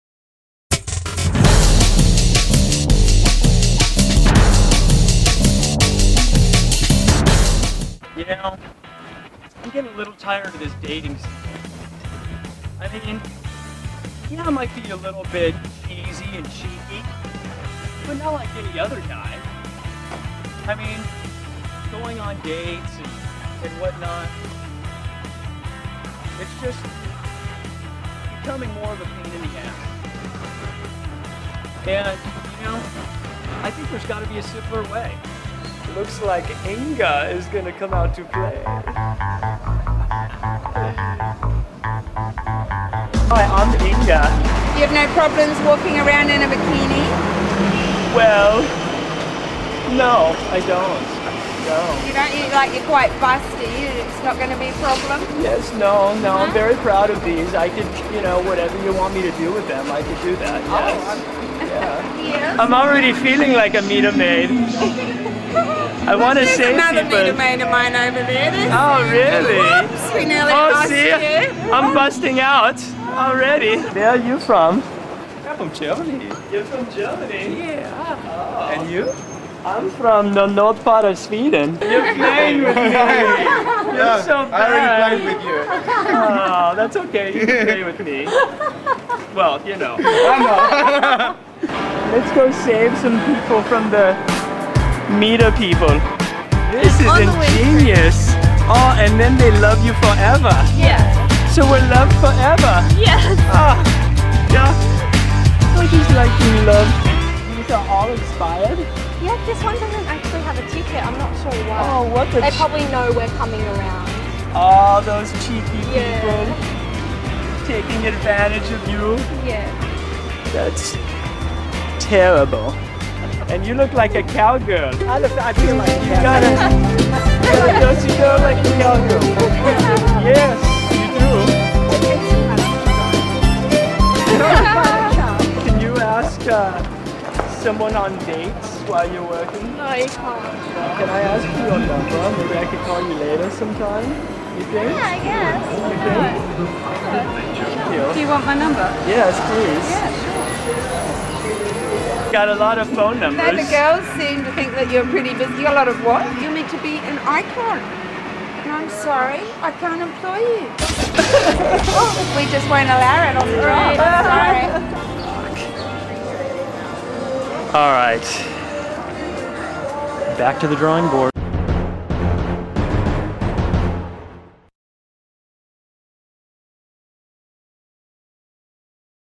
Inga Netherknockers Begins
Davin Powers creates his alter ego, Inga Netherknockers to get women.